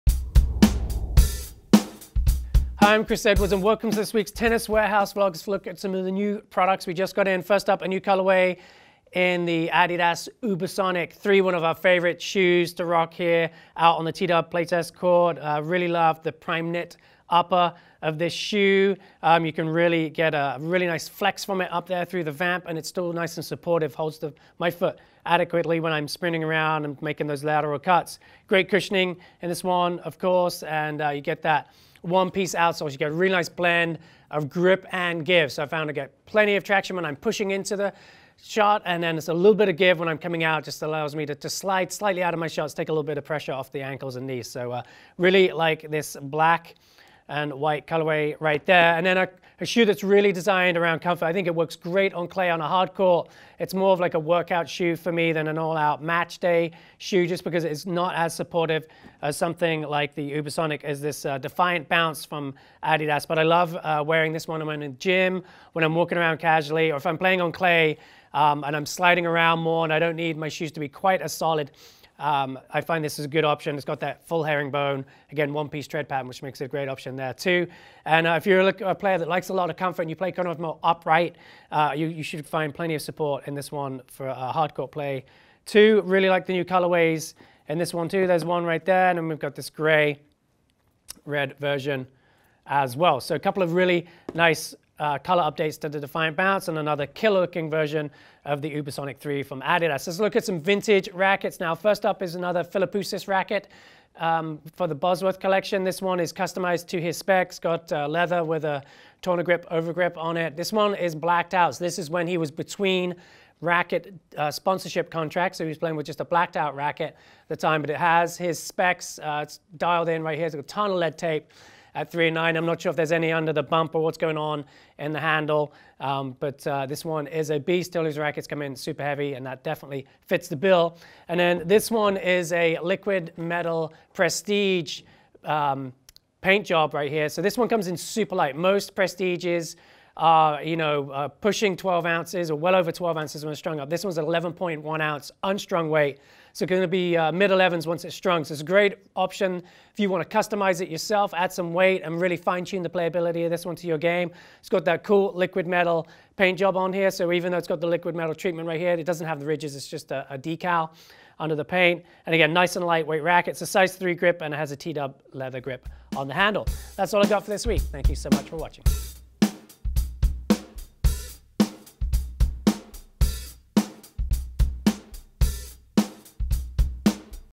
Sneak peek new adidas tennis shoe colors! + Rare Bosworth Racquets -- VLOG #620
Sneak peek new adidas shoe colors! + Rare Bosworth Racquets -- VLOG #620

Take a sneak peek at some of the new colors of adidas tennis shoes!

Plus, a HEAD racquet from Mark Philippoussis as well as another Head classic racquet we love!